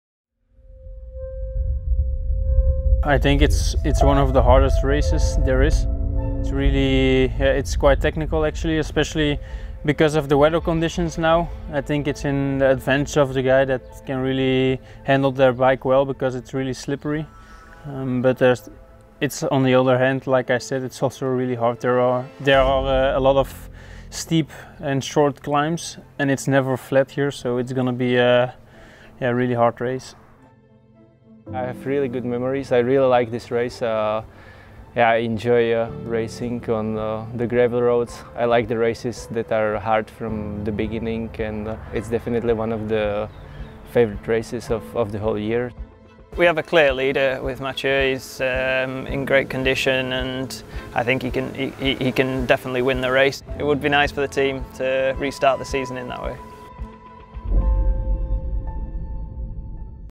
Strade Bianche - Recon | Alpecin-Fenix
First time on the white roads of Tuscany, the recon of one of the most iconic races in the World of Cycling.